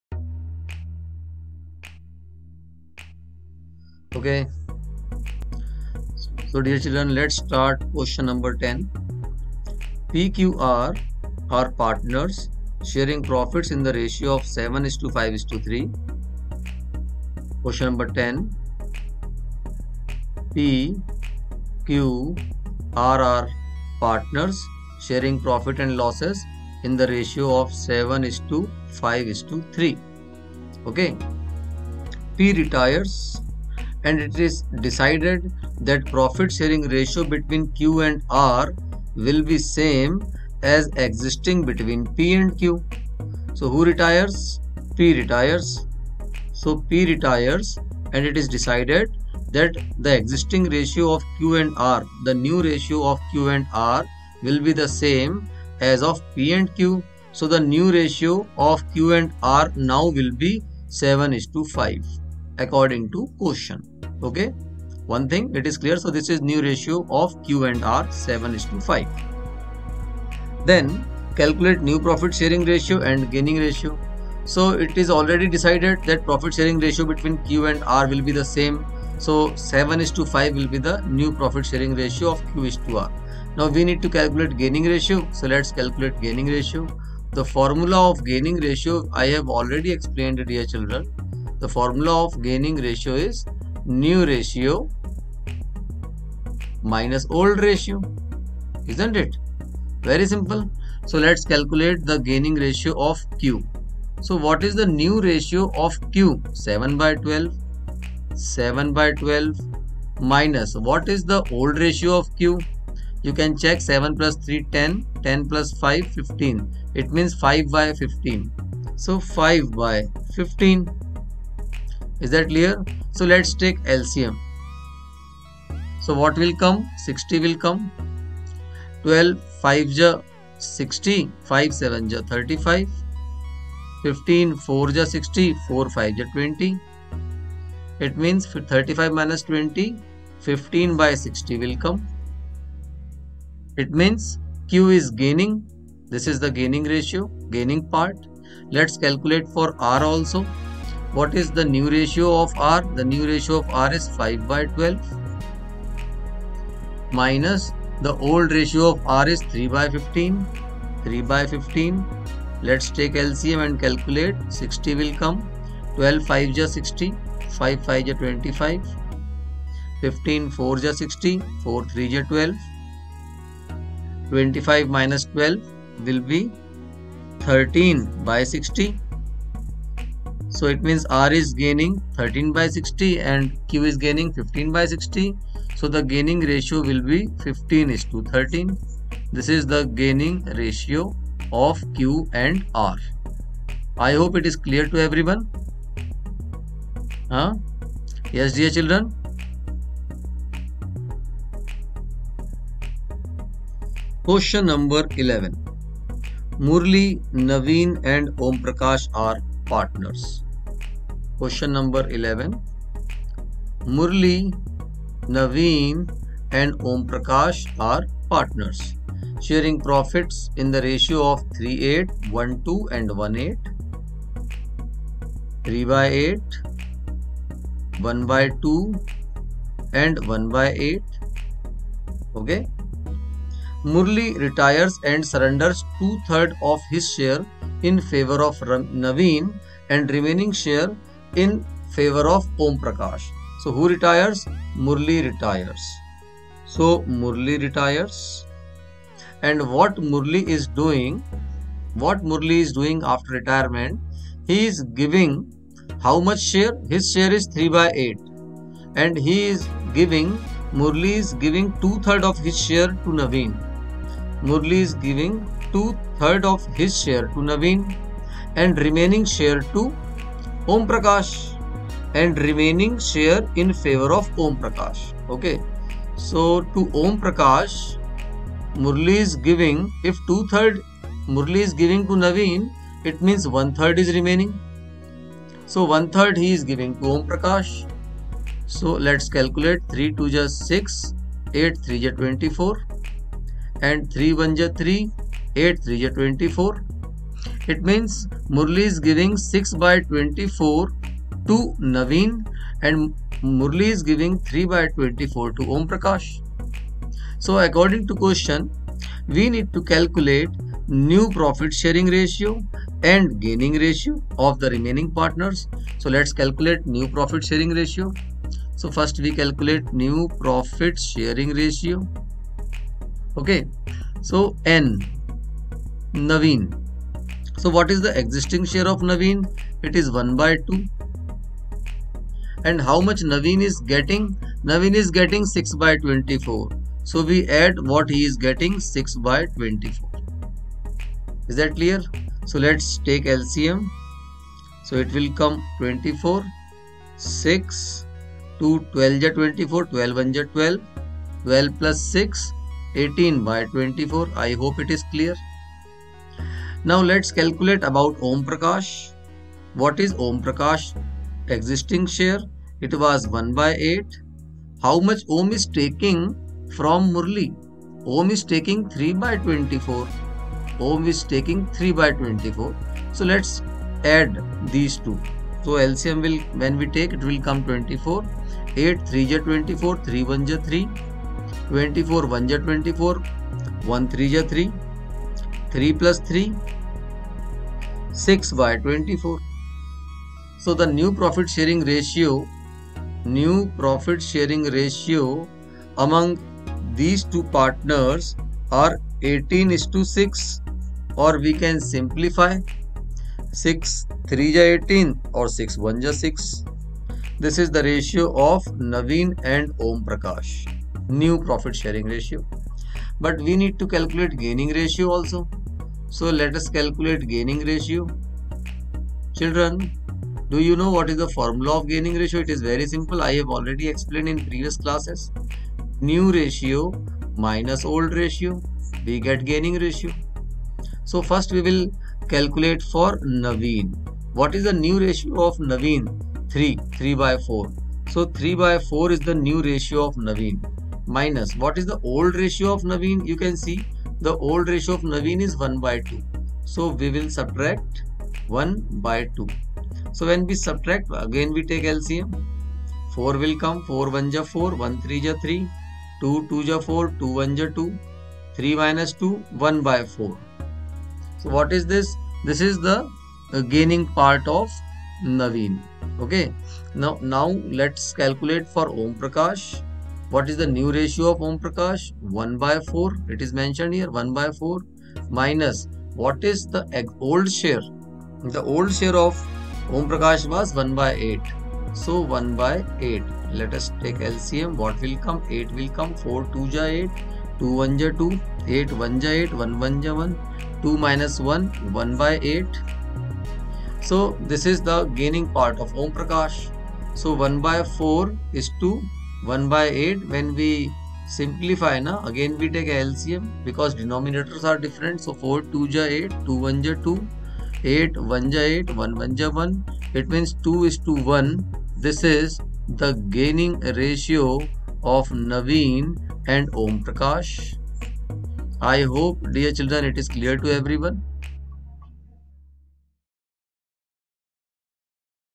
Q.No 10 and 11 | Retirement of a Partner | Class 12th | TS Grewal Solutions | 2020-21
TS Grewal Solutions, Retirement of a Partner
Calculation of New profit sharing ratio and gaining ratio.
In this question you will learn how to calculate new profit sharing ratio and gaining ratio.
It is very important to learn topic new profit sharing ratio and gaining ratio.